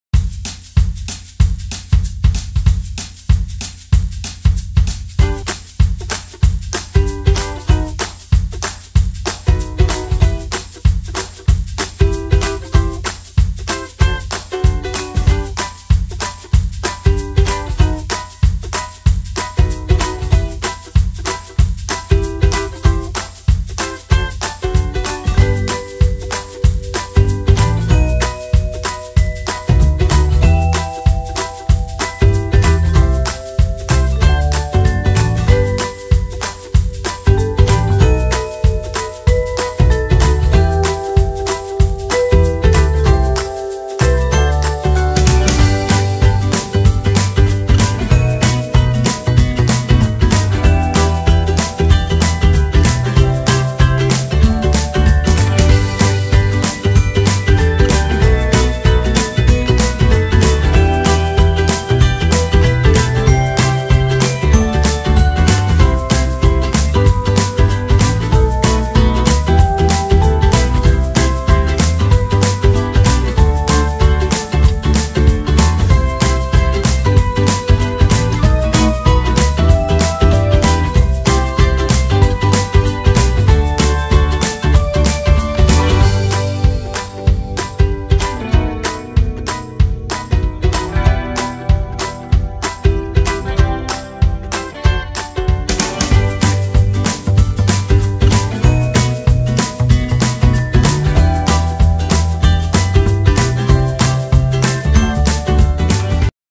Tuňák s chřestem a těstovinami
Inspirujte se s Moje barevná kuchyně a svěžím receptem pro letní romantickou večeři s tuňákem Rio Mare!🍴Tuňák s chřestem a těstovinami vykouzlí atmosféru středomořské pohody v každé kuchyni…